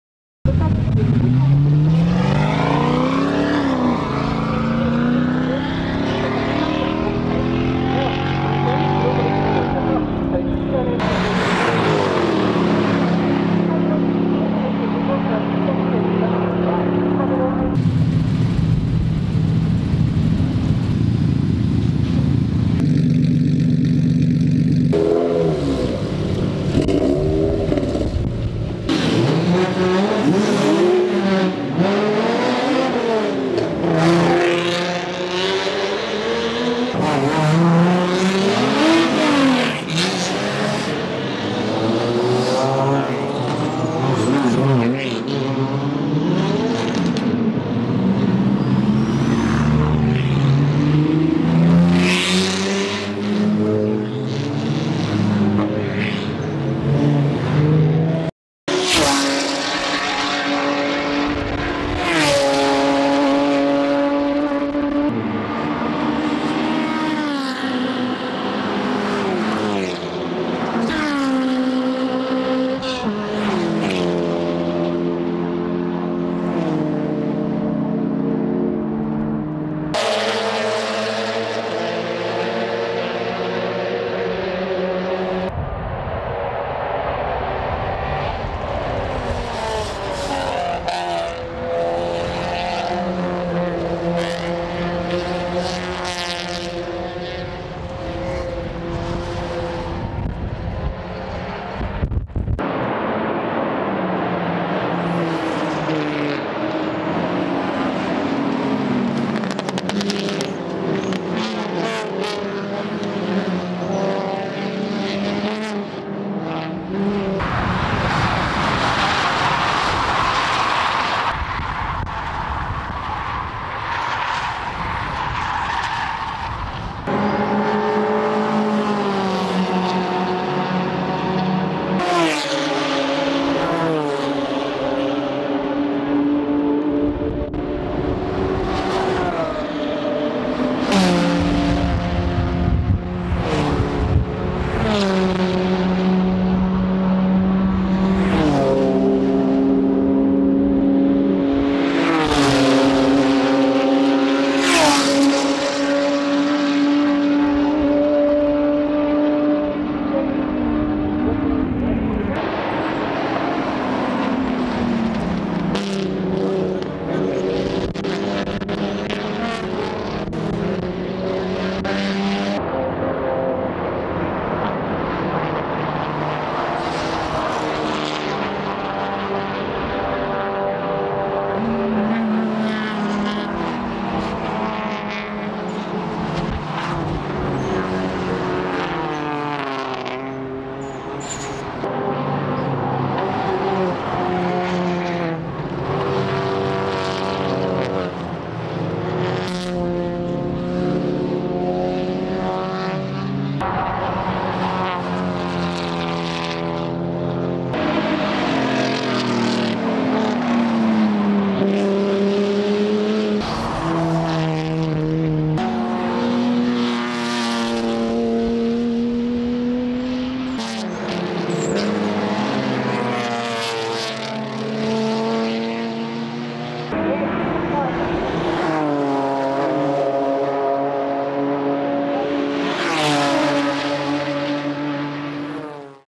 넥센타이어 스피드레이싱 1R 레이스카 사운드 - NEXENSPEEDRACING RACE SOUND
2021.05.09
넥센타이어스피드레이싱 1R 태백스피드웨이
레이스카 영상 & 사운드 일부 모음영상입니다.

참가하신 선수 및 대회관계자,오피셜분들
모두 수고많으셨습니다.